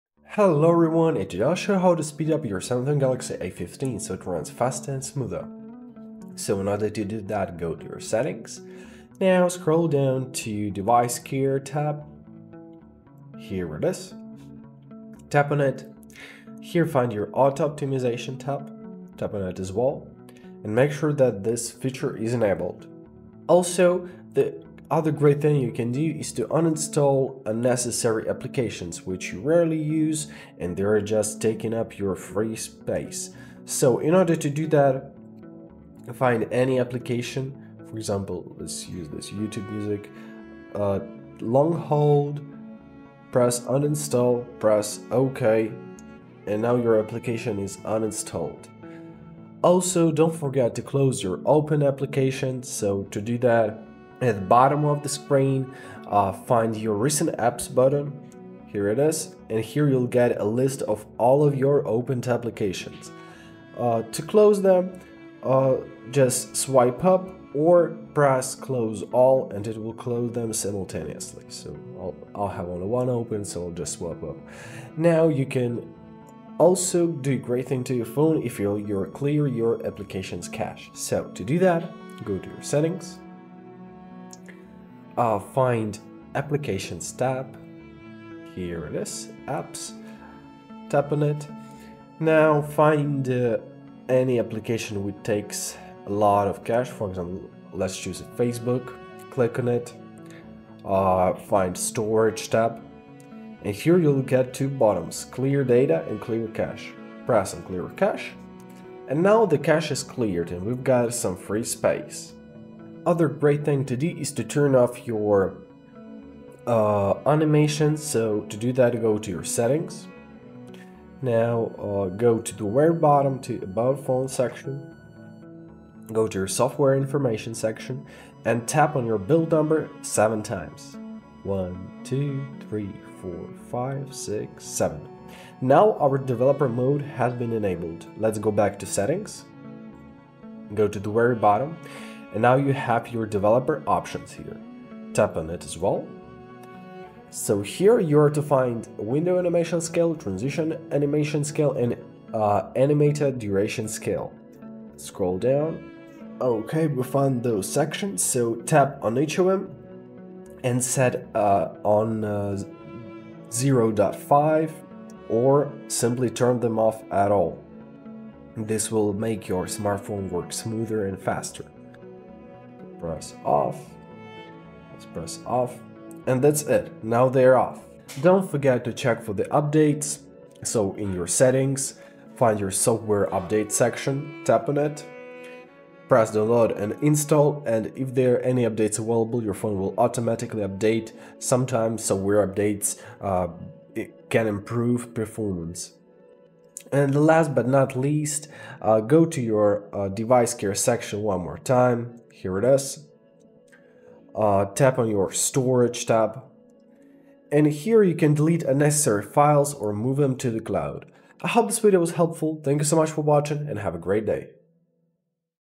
How to Make Your Samsung Galaxy A15 5G Faster
Is your Samsung Galaxy A15 5G running slow or lagging? In this video, I’ll show you how to make your phone faster step by step. You’ll learn how to clear cache and unnecessary files, close background apps, adjust animations, and enable features like RAM Plus to improve speed. I’ll also share battery and storage tips to keep your phone performing like new.

This video also covers the following topics:
👉 Speed up Samsung Galaxy A15 5G easily
👉 Clear cache & delete unused files
👉 Use RAM Plus & performance settings
👉 Optimize battery and storage for better speed